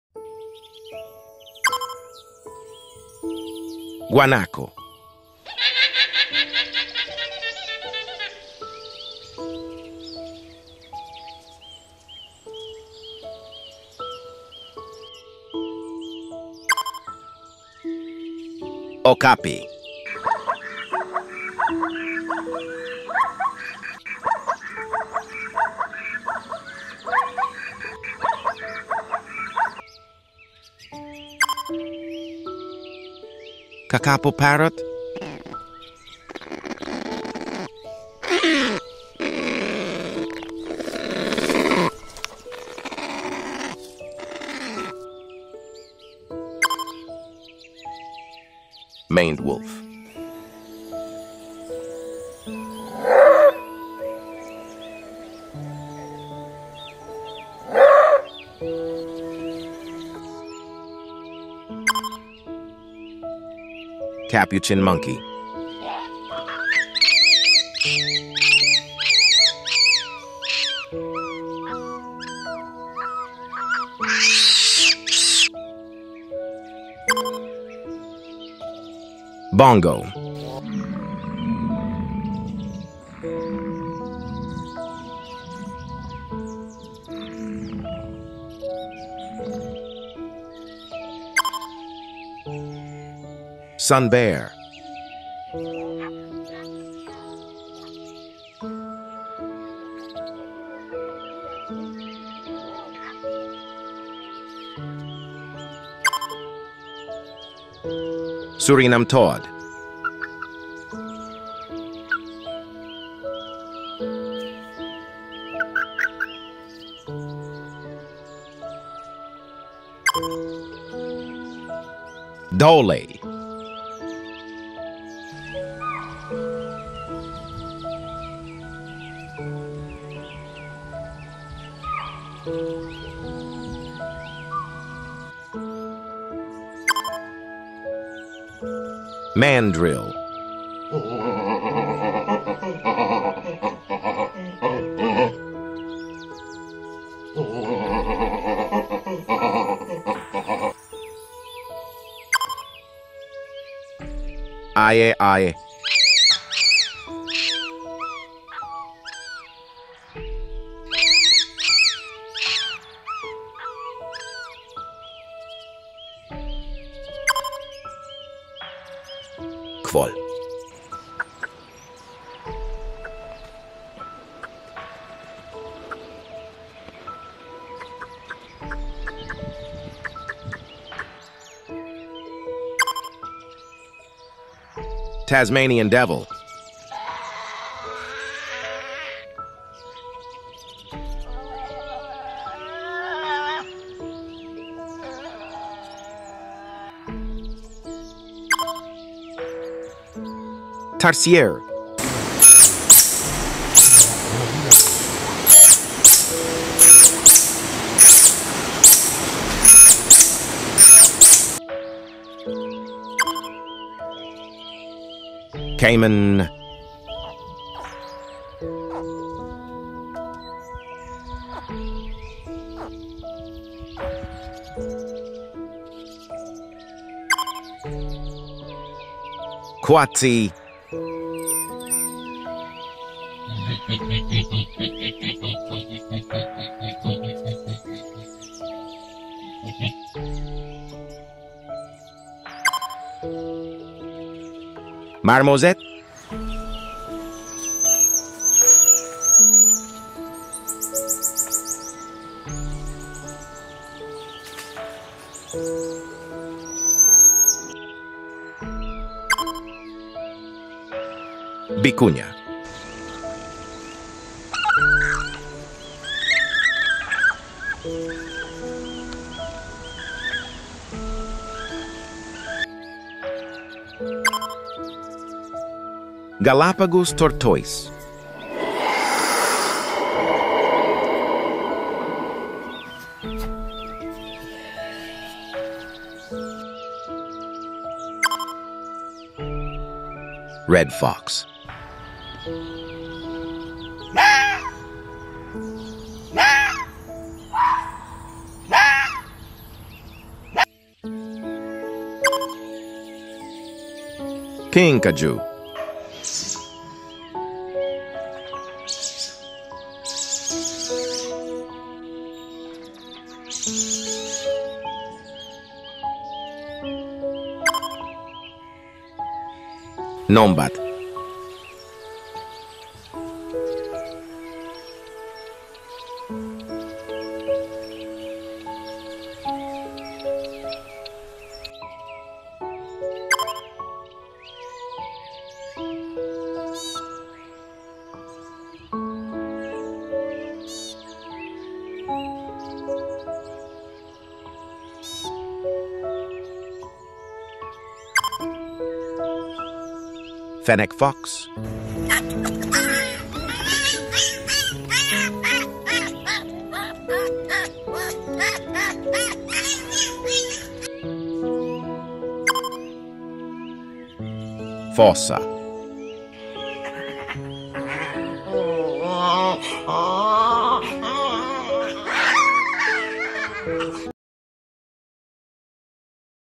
Explore the Amazing World of Animals for Kids | Fun and Learning Together
🌟 Unveiling the Marvels of the Animal Kingdom: Names and Sounds! 🐾🔊

Embark on a captivating journey with our latest video featuring an array of fascinating animals and their unique sounds. From the energetic chirps of pikas to the fierce growls of Tasmanian devils and the harmonic calls of peafowls, get ready for a delightful audiovisual experience. Listen to the haunting sounds of aye-ayes, the gentle hums of vicuñas, and the mysterious calls of tarsiers. Explore the rhythmic beats of capuchin monkeys, the adorable squeaks of fennec foxes, and the intriguing croaks of Surinam toads. From the playful chatter of kinkajous to the buzzing of numbats, and the vibrant squawks of Kakapo parrots, this video is a celebration of the diverse voices in the animal kingdom. Join us in discovering the names and sounds of these extraordinary creatures. 🌿🎥🐾

A big thank you for spending your time with our video! 🙏